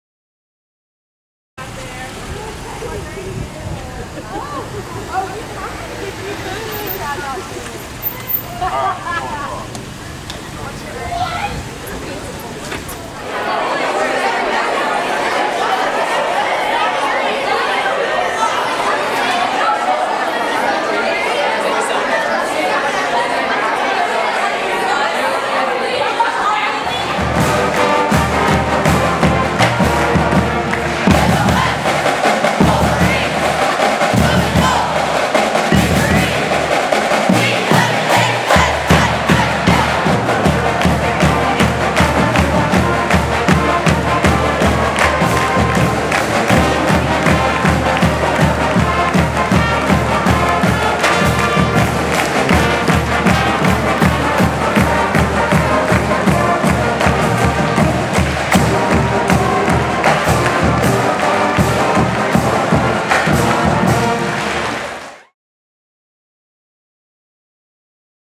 First Day at Bentonville West High School
Students arrive for the first day of class as the new Bentonville West High School in Centerton. The school will begin it's first year with 9th-11th grade students.